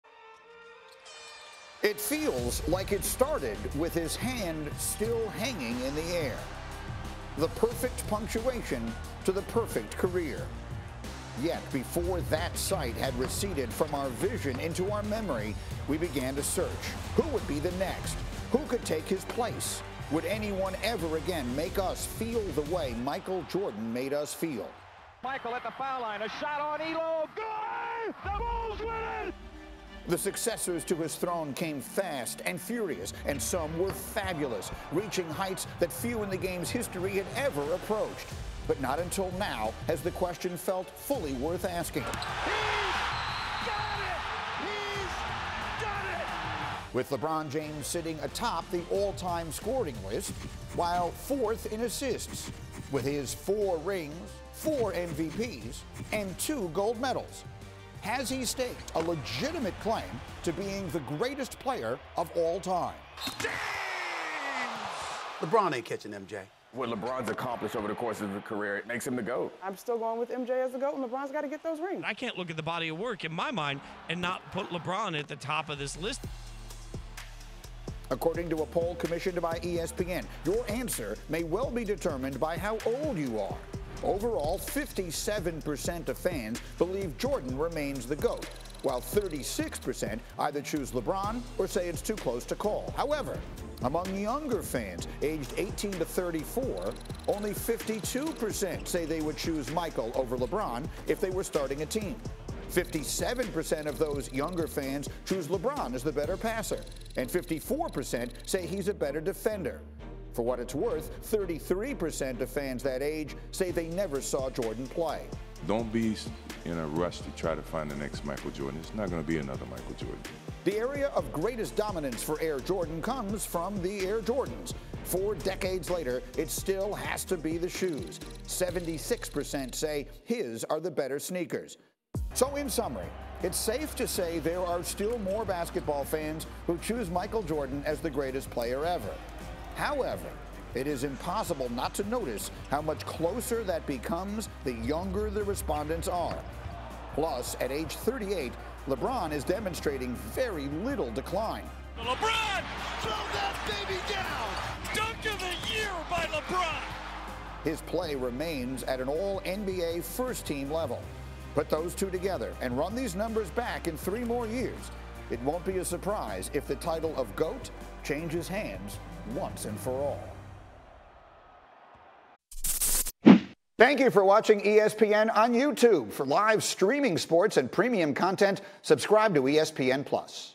The NBA GOAT Debate 🐐 Michael Jordan vs. LeBron James
Mike Greenberg takes a look at the NBA GOAT debate between Michael Jordan and LeBron James.